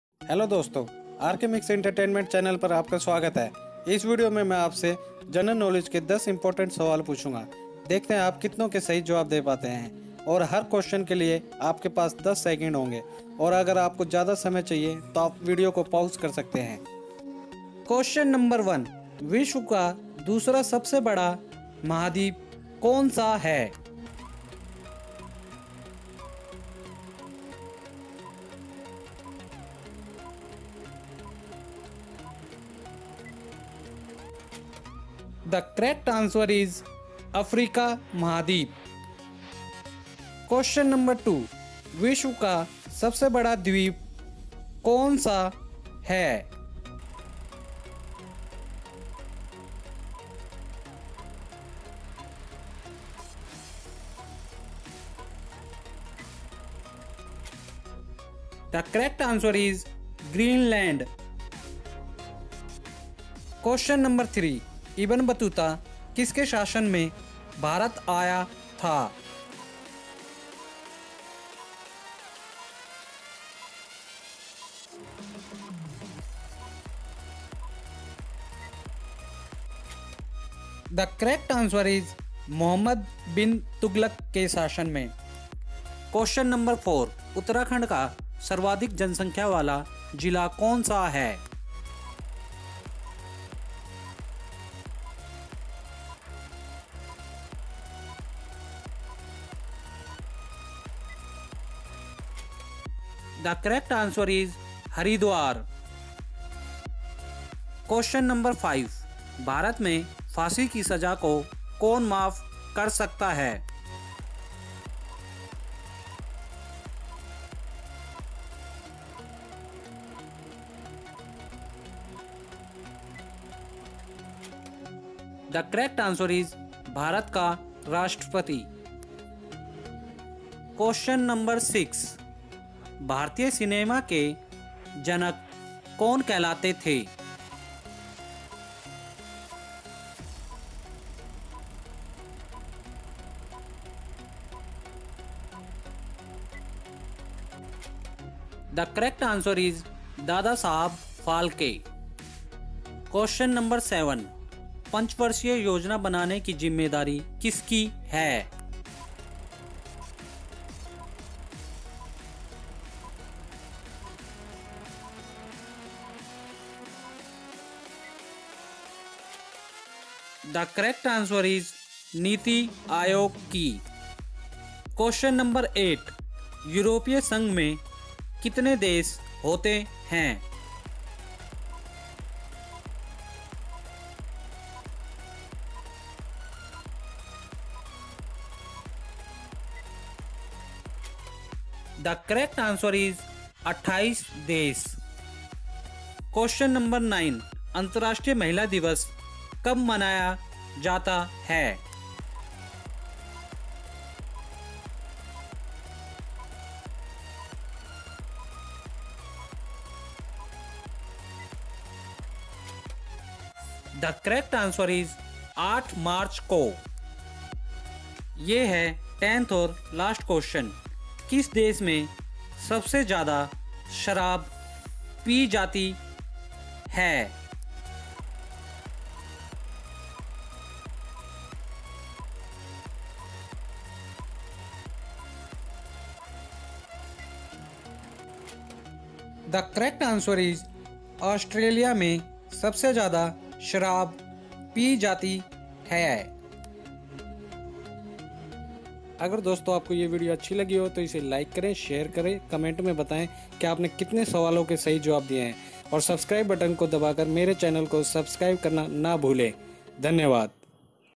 सामान्य ज्ञान के 10 बेहद जरूरी सवाल || 10 Most Important Questions Of General Knowledge
This video asks 10 questions of general knowledge which is mandatory for every child who preparing for competitive exams.
Let's see how many you confirmed.